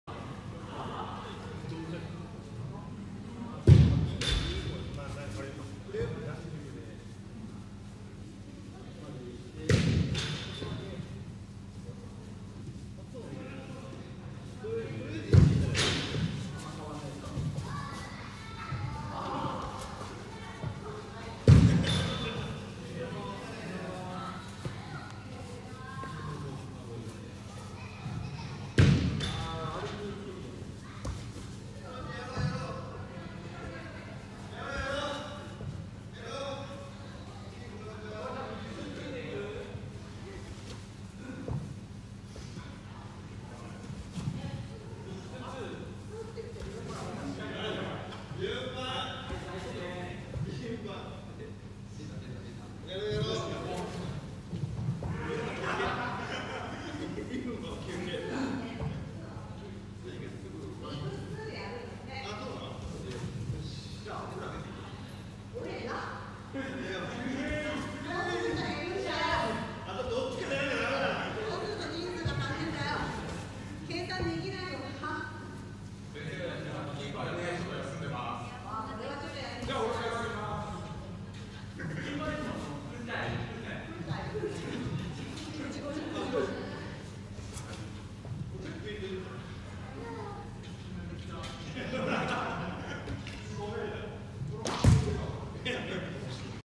ワンタッチツルキ練習2
2021/11/20（土）
・これほんとむずい。まだ糸口も掴めてない感じ。数をこなして感覚掴む。